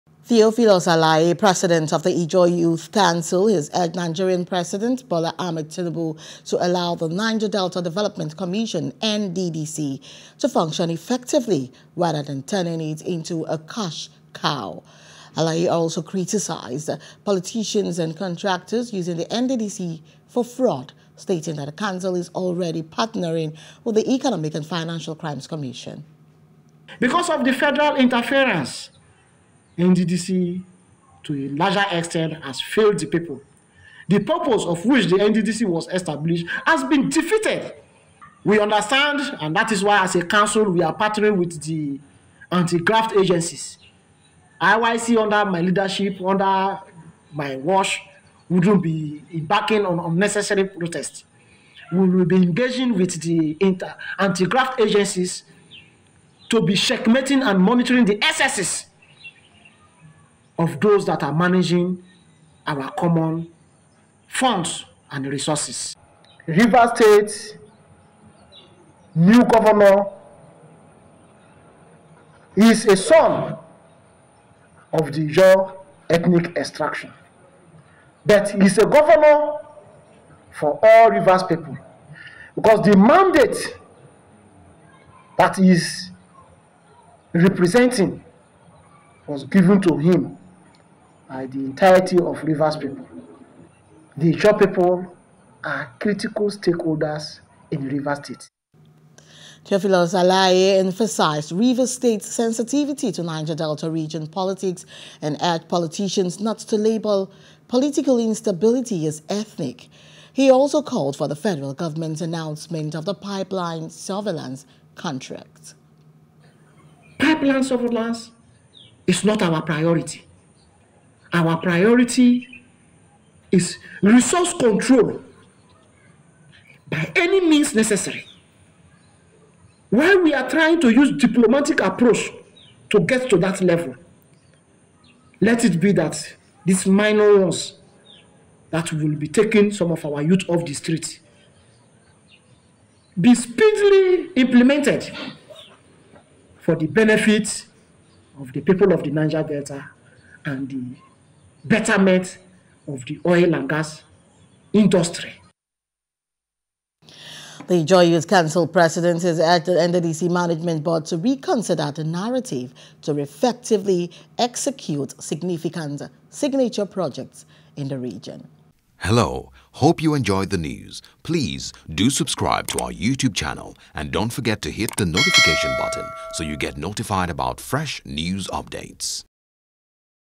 Governance: Politicians And Contractors Using The NDDC As A Ground For Fraud Your Days Are Numbered
Theophilus Alaye, president of the Ijaw Youth Council, has urged Nigeria's President Bola Ahmed Tinubu to allow the Niger Delta Development Commission (NDDC) to function effectively, rather than turning it into a cash cow. Alaye also criticized politicians and contractors using the NDDC for fraud, stating that the council is already partnering with the Economic and Financial Crimes Commission.